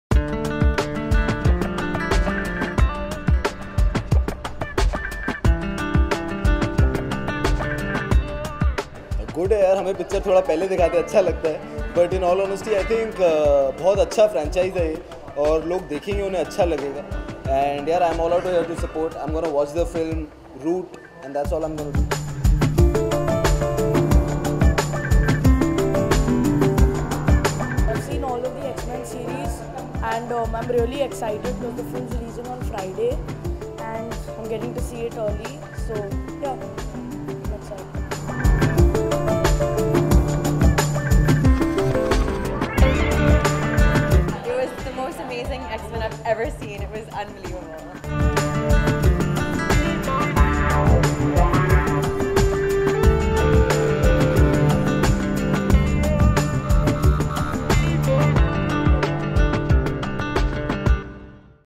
Arjun Rampal & Abhishek Kapoor at Screening of Film X MEN
The event was the special screening of the upcoming action sci-fi movie 'X-Men:Days Of Future Past'. At the screening were present Arjun Rampal with wife Mehr and very good friend Susanne Roshan, director Abhishek Kapoor, Prateik Babbar, Dino Morea, 'Kick' actress Jacqueline Fernandez and 'Hawaa Hawaai' star Saqib Saleem. For more info watch this video...